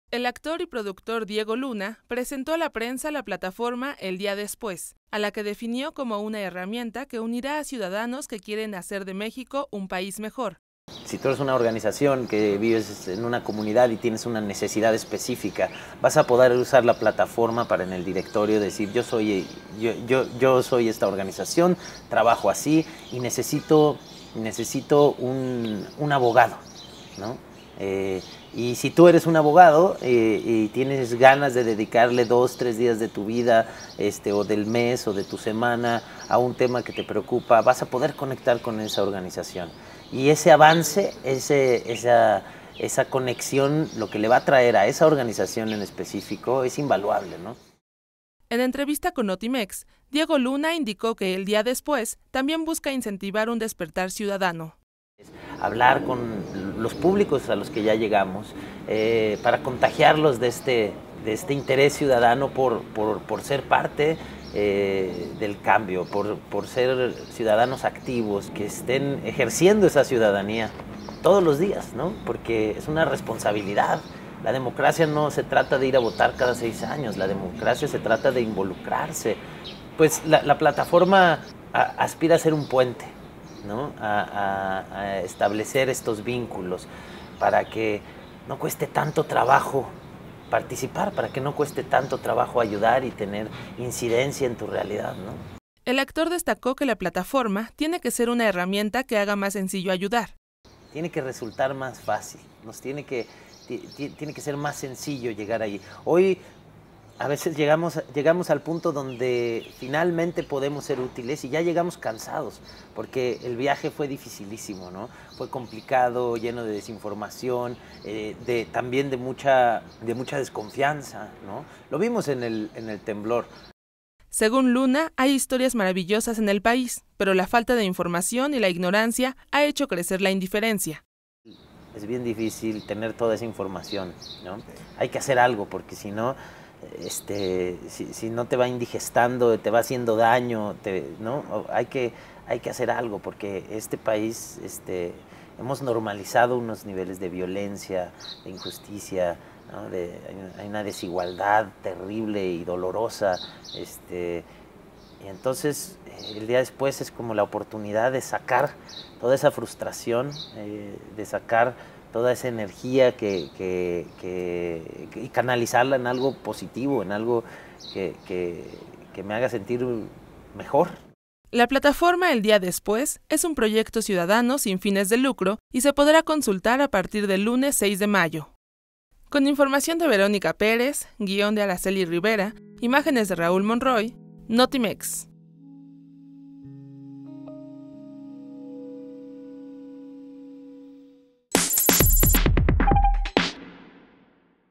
“El Día Después”, para ciudadanos que buscan un México mejor
El actor Diego Luna explicó que la plataforma será un puente que unirá a todas las asociaciones civiles para apoyar causas ciudadanas